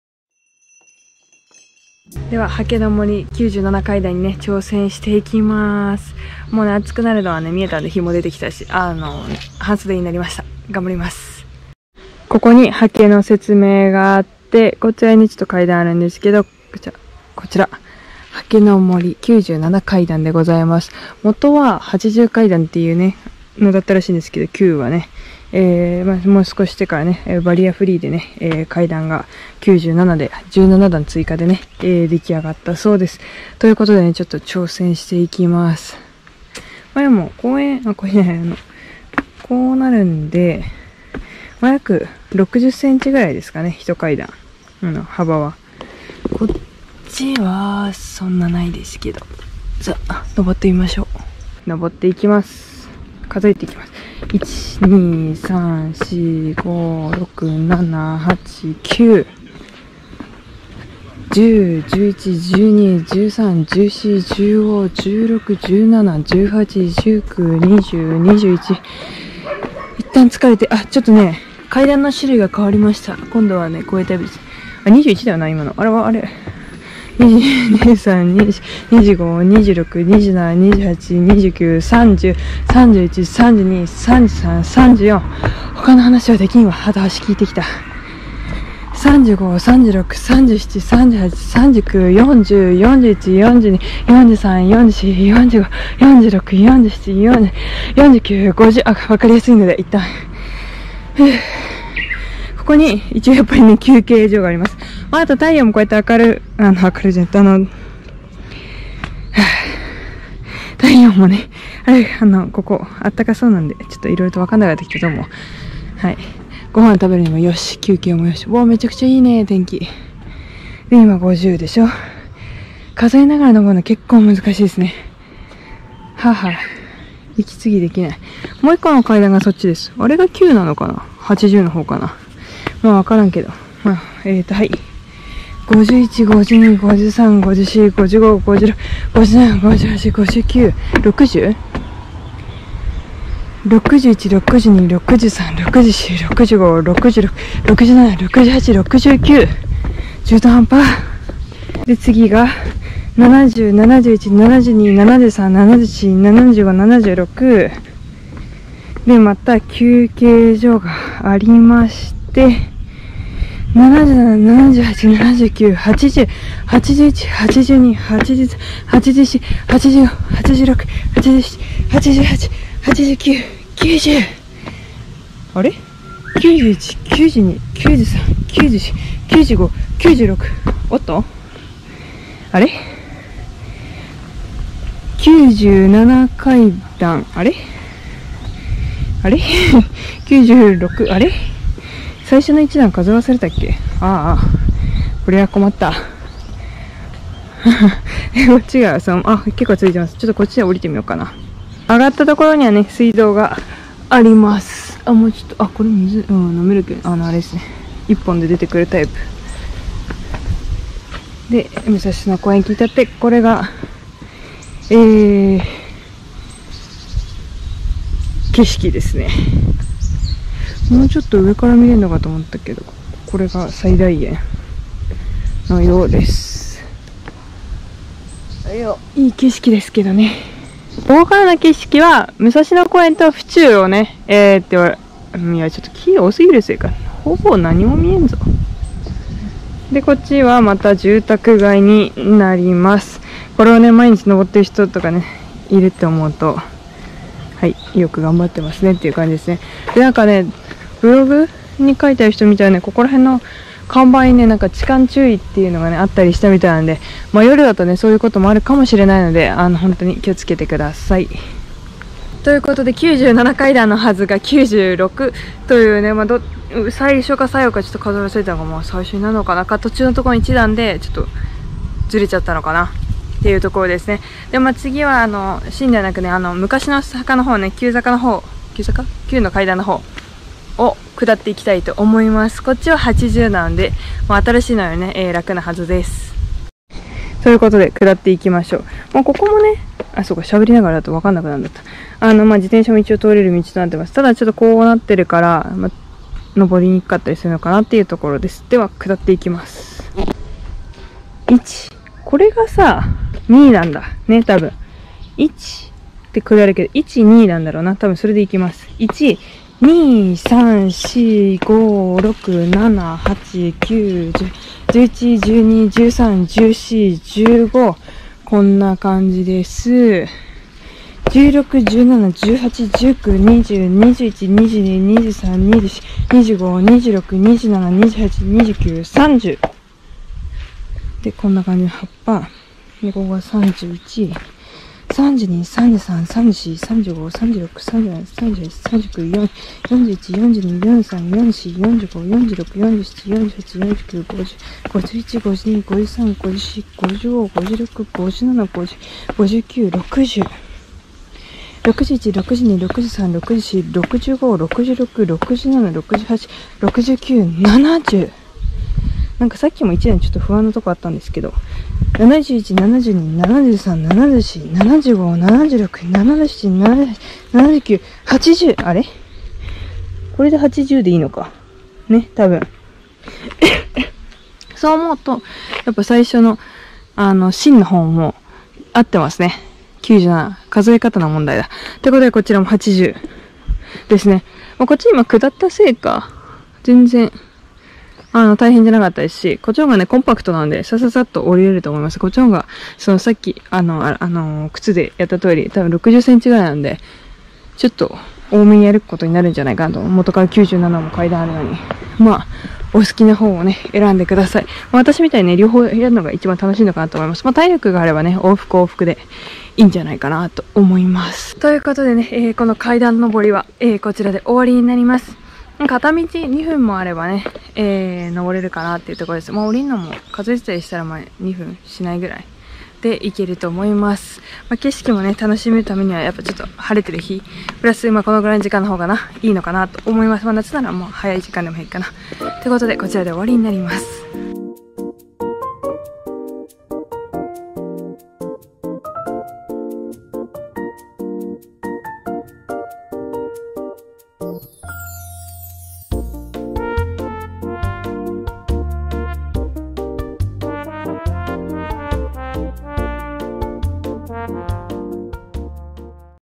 【はけの森97階段】に挑戦！武蔵小金井散歩！実況
徒歩旅が好きなすさなです🐶

今回のすさな散歩は、
東京小金井市の武蔵小金井駅にある
武蔵野公園近くのはけの森97階段に挑戦しました🚶🏽‍♀️
Musashino park Tokyo-koganei-shi

今後は、関東圏を歩き回ります😌
ここからここまで歩いてほしい！
という場所があったらコメントください🌈

【SUZURI】オリジナルグッズ販売🎨

【LINE スタンプ】3種類あります🐎

【スペイン語 Español チャンネル】日本の色々を紹介🐒